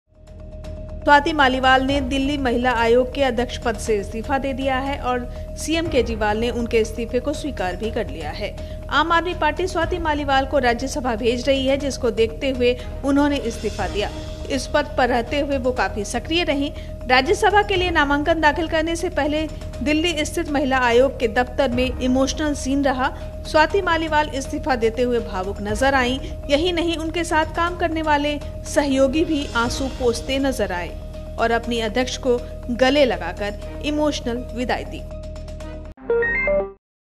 DCW अध्यक्ष पद से स्वाति मालीवाल का इस्तीफा मंजूर, रोते हुए कहा बाय
➡️स्वाति मालीवाल का इस्तीफा मंजूर
➡️DCW अध्यक्ष पद से दिया इस्तीफा
➡️राज्यसभा के लिए स्वाति ने भरा नामांकन